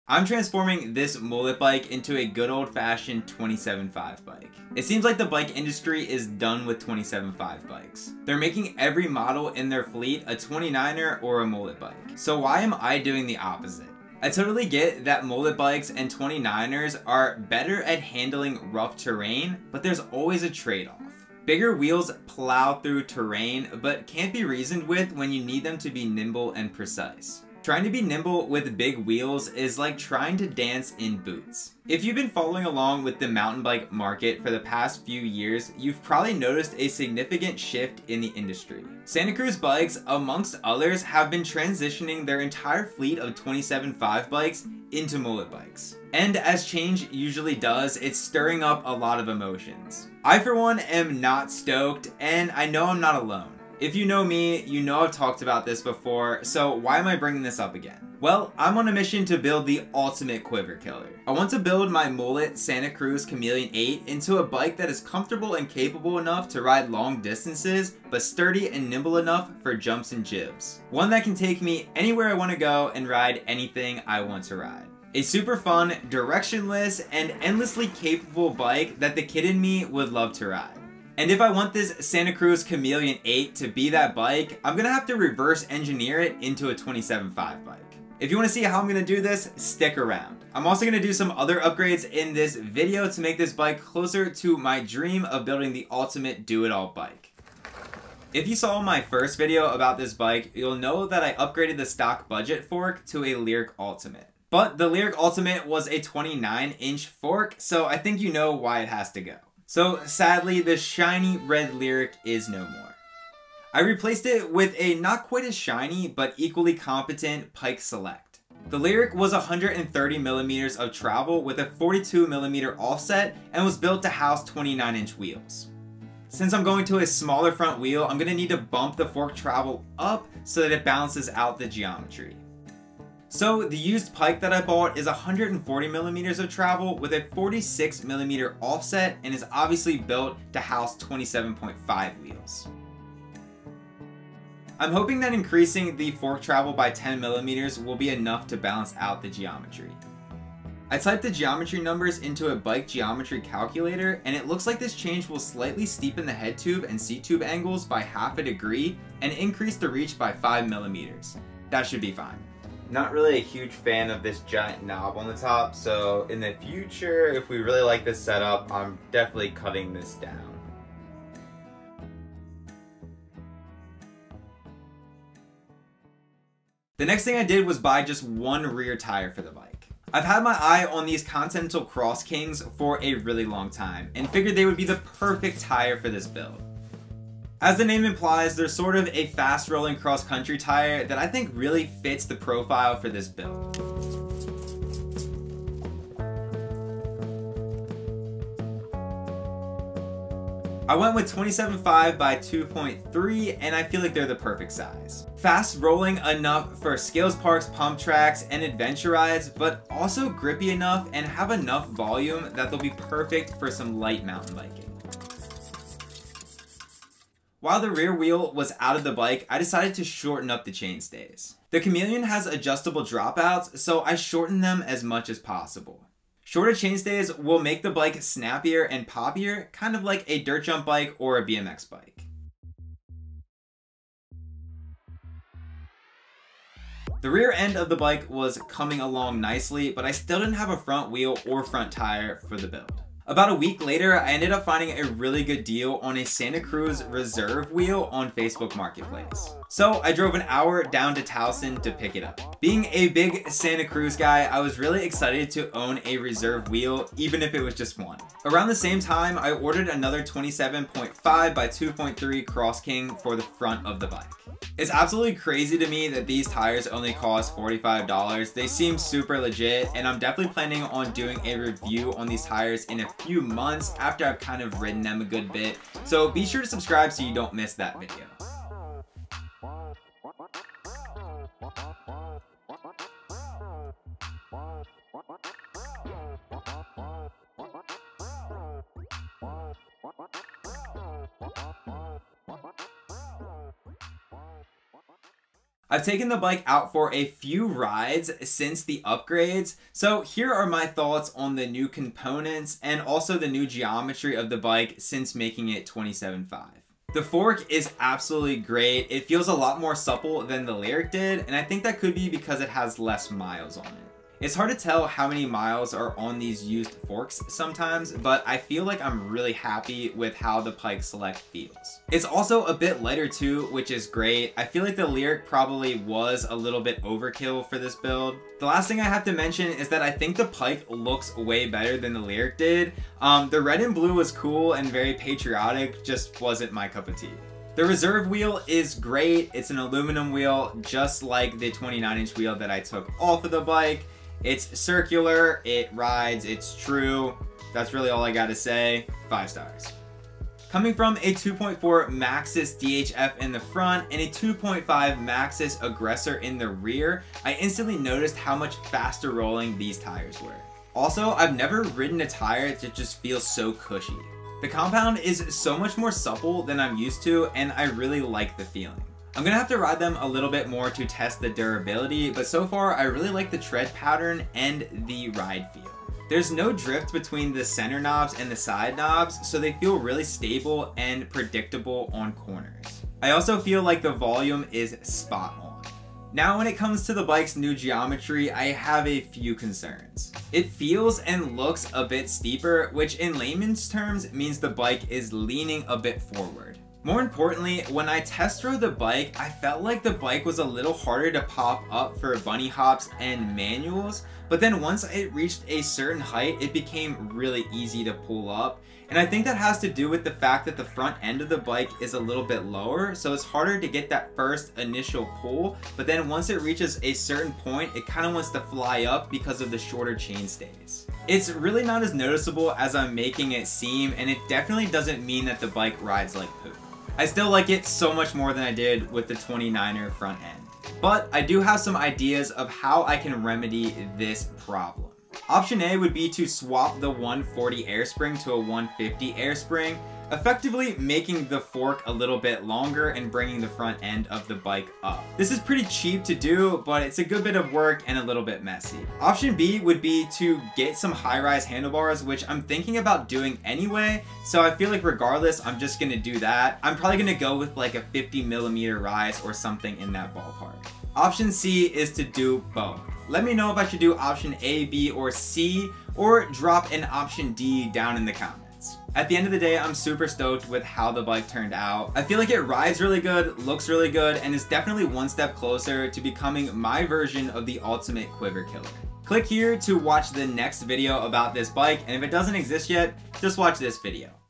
Santa Cruz Ruined the Chameleon... So I Fixed It.
In this video I am converting my mullet Santa Cruz Chameleon 8 mountain bike into a 27.5 bike. The new Chameleon is only available in mullet or 29er, so I'm taking the liberty of creating a 27.5 option!

I’m on a mission to build the ultimate do-it-all quiver killer mountain bike! This is the second video in this series.

It's free to do and means a lot to us!